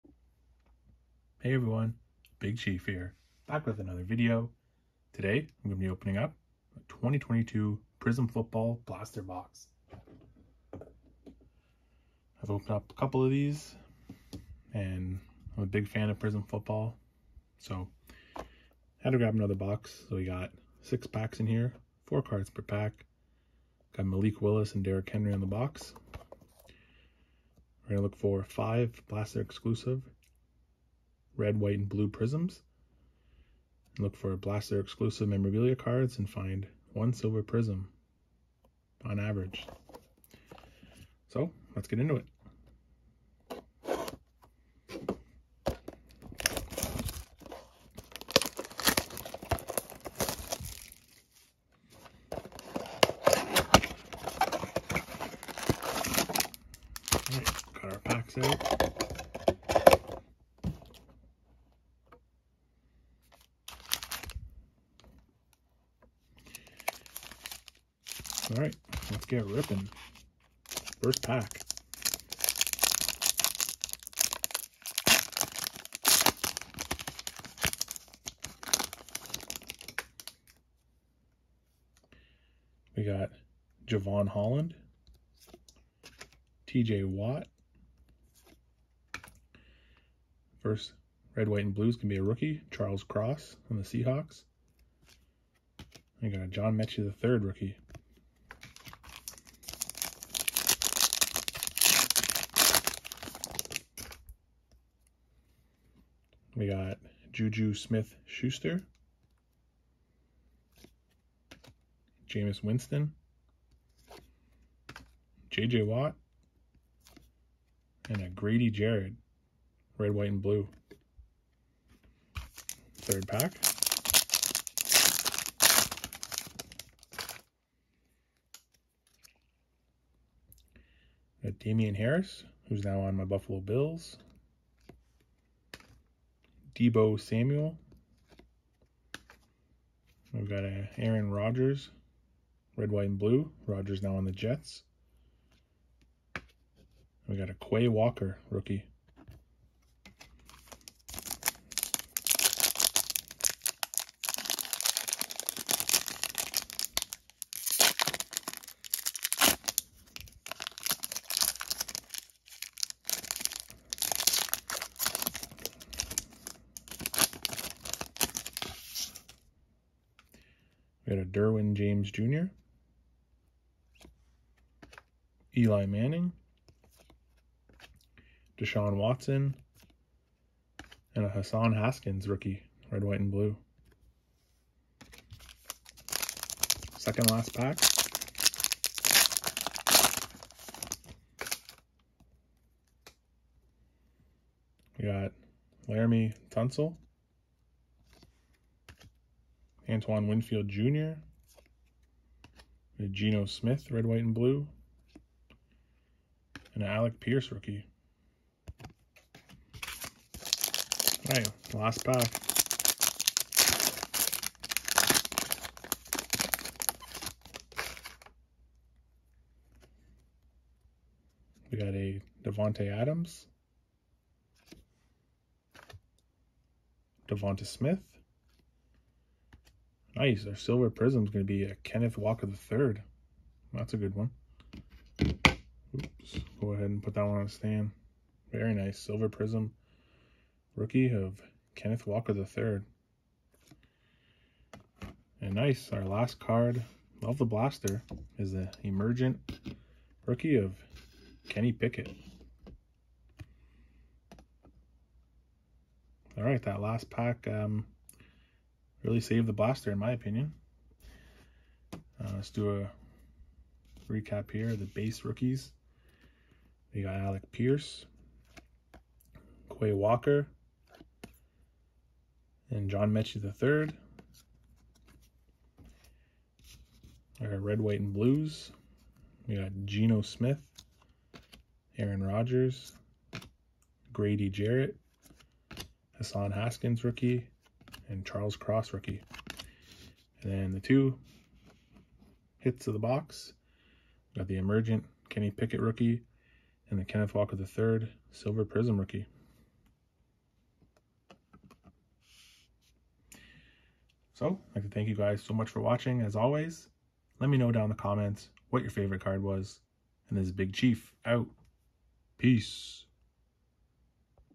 2022 Prizm Football Blaster Box Opening
Today I am opening up a 2022 Prizm Football Blaster Box. More pack openings coming soon!